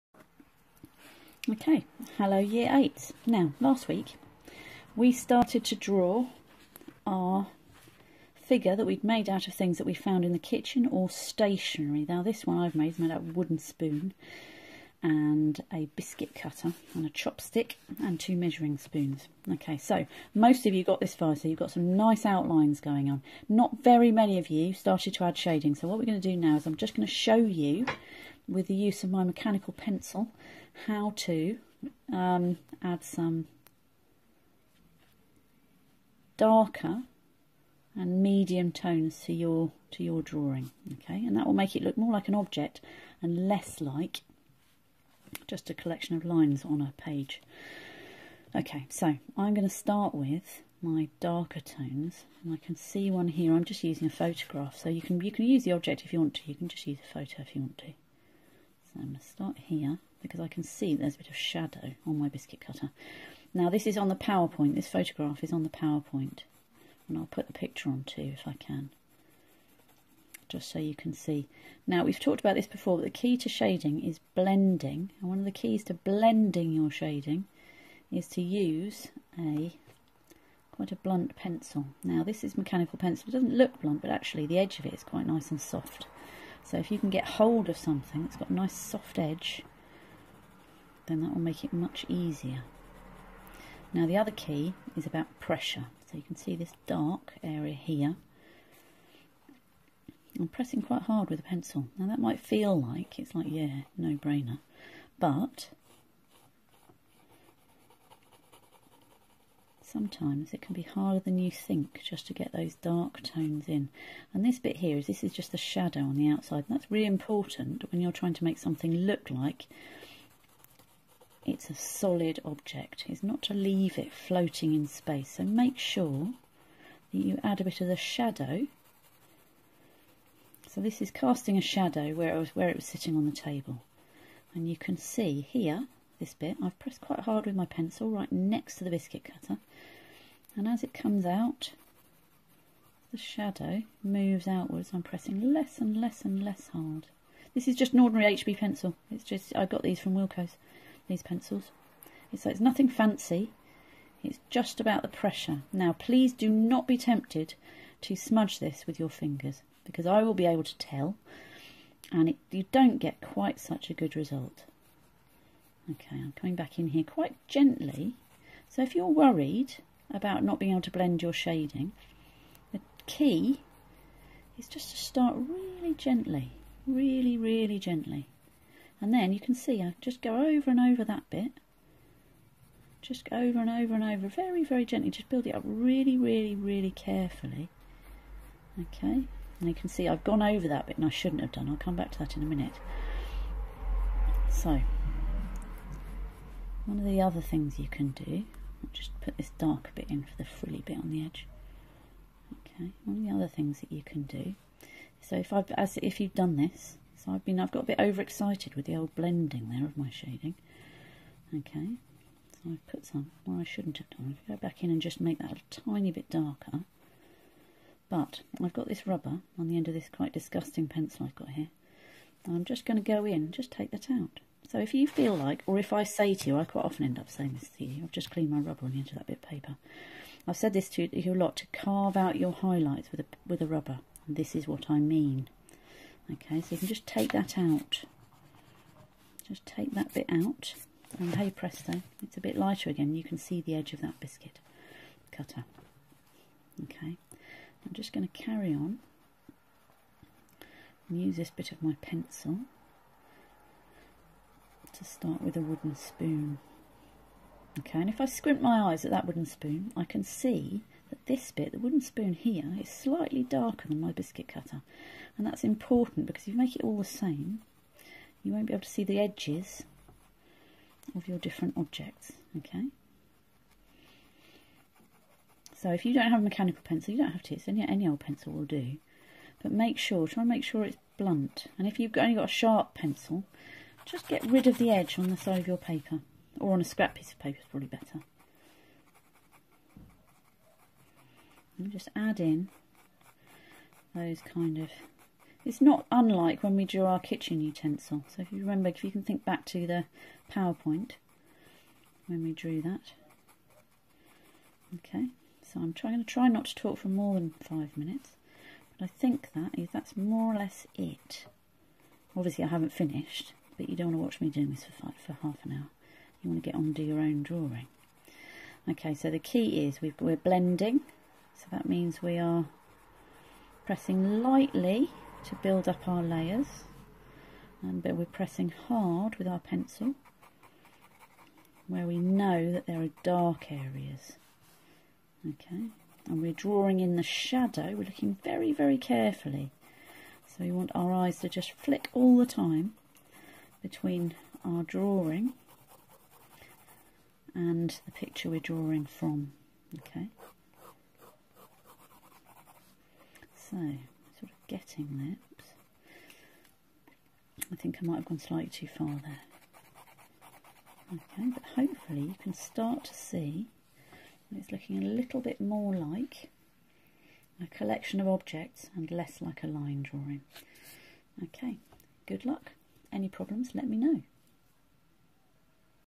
Adding shading to your kitchen implement sculpture
A tutorial to show you how to add dark and medium tones to make your objects look 3D.